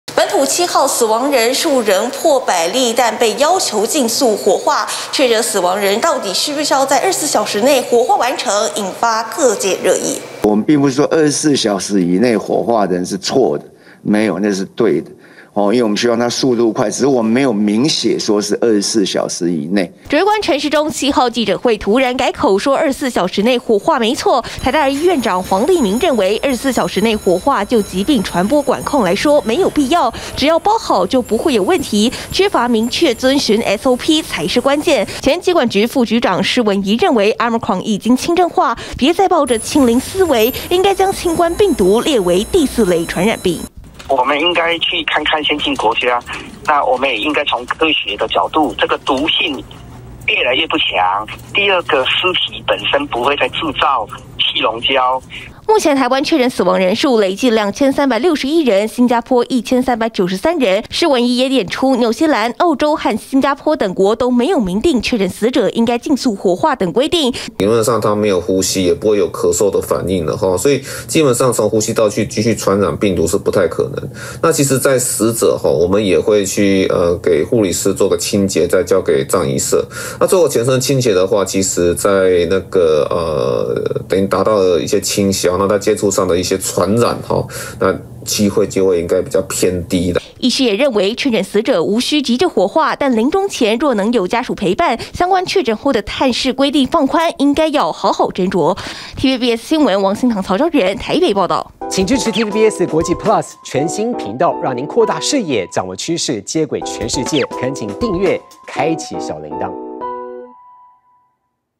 24hr內火化亡者沒SOP 專家：新冠應降四類｜TVBS新聞
確診死亡人數連續一週都破百例，但死者究竟需不需要在24小時內完成火化，指揮官 陳時中昨(7)天記者會上突然改口說，24小時內火化沒錯，只是沒有明文規定，專家則認為，應該明定SOP，或將新冠病毒降為第四類傳染病，才能避免爭議再發生。